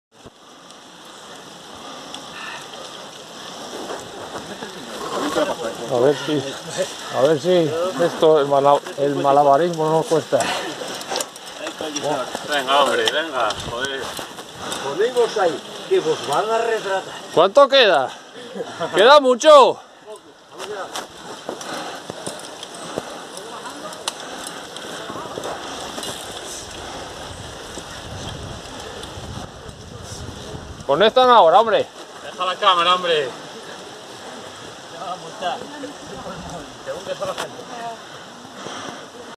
Kedada Aniversario 2019 (La Mozqueta, 1)
Video en la tercera ascensión de la Kedada Aniversario 2019, la Mozqueta.